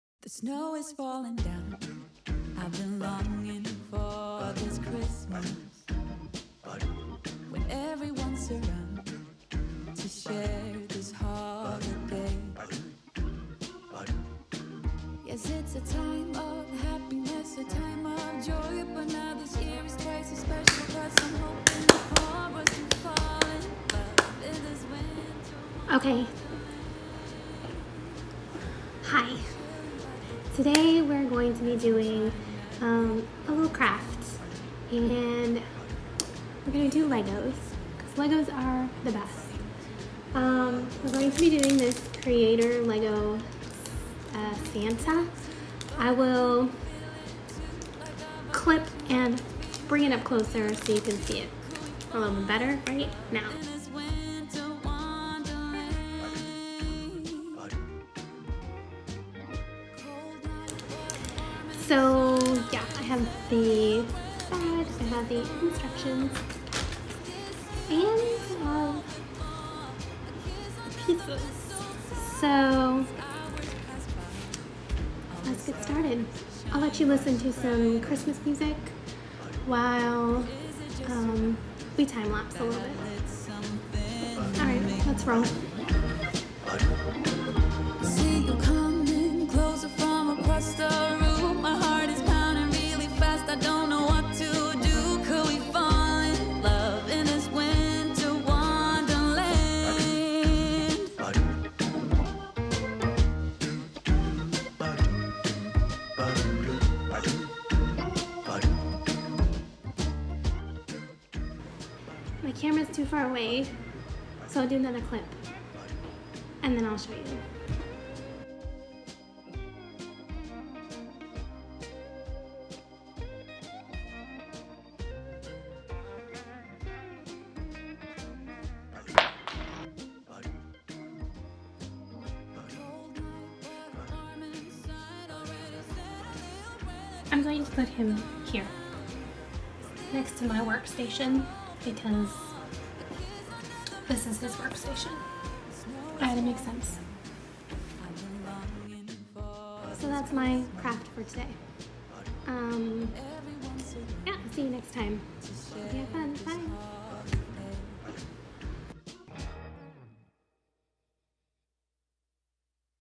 LEGO Santa 2020 // LC DIY
► LEGO Santa 2020 // LC DIY In this vlog:  Bringing back an old holiday classic from 2017!

►  Question of the week:
What crafty or creative things do you do over the holidays?

      Lisacmillward
(or my old blog those were
      the days...)

Music/Editing: Filmora
Cameras used: iphone 7, GoPro Hero+
Audio: Rode SmartLav +
Extension Cable
Discrete Gel Mounting Pads